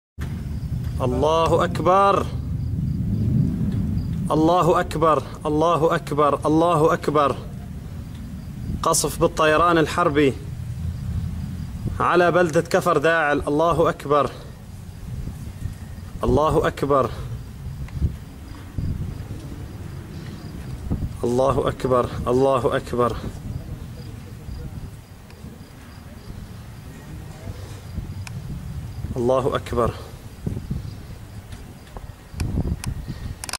شام ريف حلب كفرداعل تصاعد أعمدة الدخان جراء القصف العنيف 22 7 2013
مراسل شبكة شام أبو المجاهد رصد تصاعد أعمدة الدخان جراء القصف العنيف على بلدة كفرداعل غربي مدينة حلب.